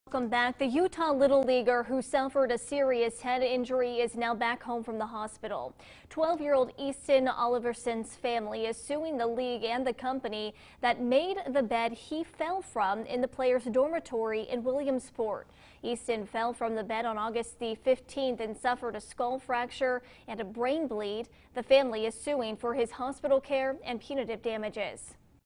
Family sues Little League over bunk bed fall, head injury
Family sues Little League over bunk bed fall, head injury. 9/21/22